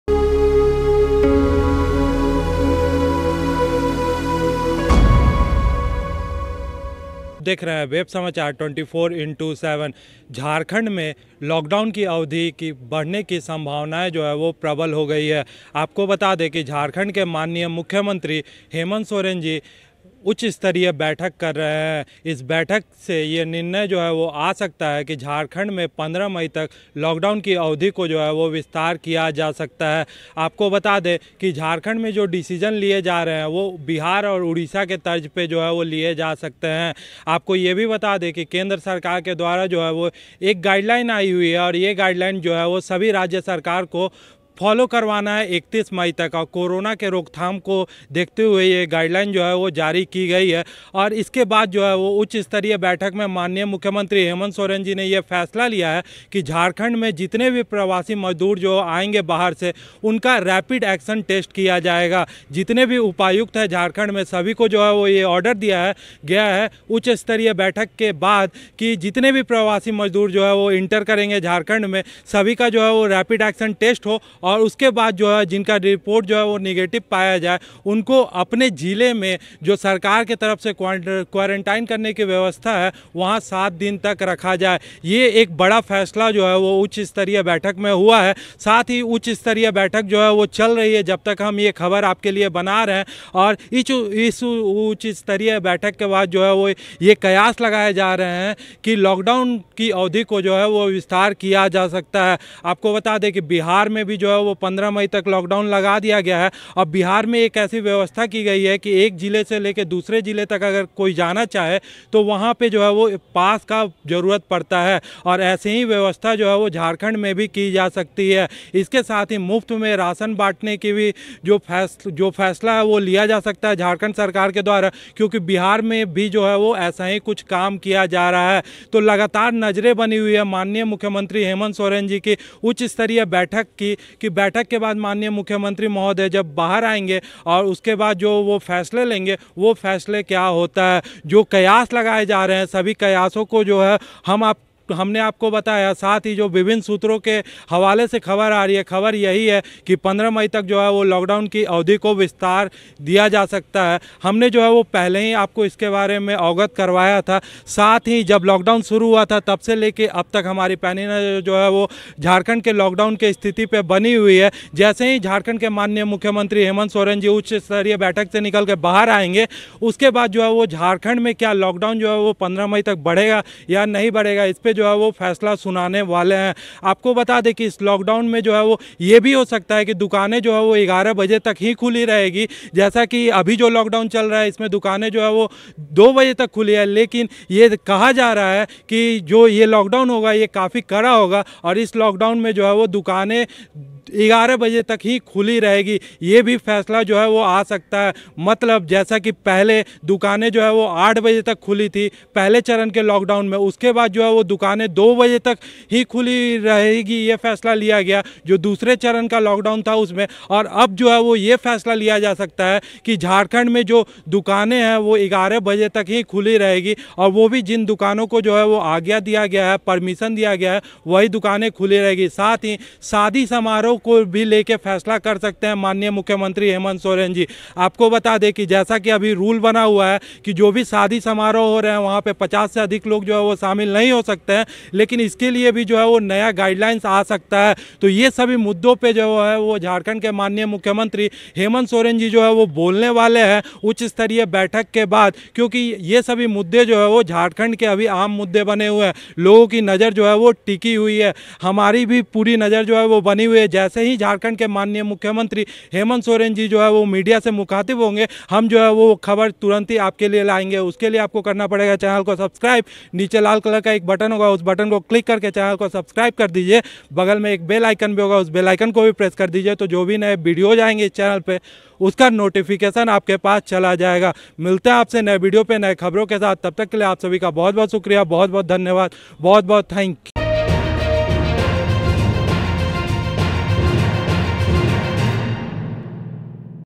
झारखण्ड में फिर बढ़ेगा लॉकडाउन, सीएम हेमंत सोरेन कर रहे है मीटिंग, लेंगे बड़ा फैसला | Web Samachar
झारखण्ड में फिर बढ़ेगा लॉकडाउन, सीएम हेमंत सोरेन कर रहे है मीटिंग, लेंगे बड़ा फैसला | Web Samachar 24x7